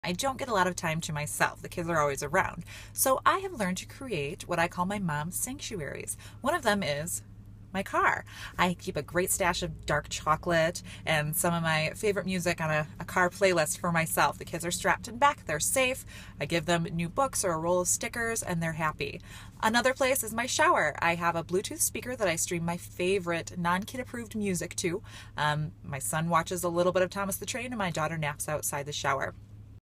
30Second Mom Video: Cheryl Leahy Shares How She Creates "Mom Sanctuaries" for Herself
30Second Mom contributor Cheryl Leahy shares how she makes "mom sanctuaries" for herself.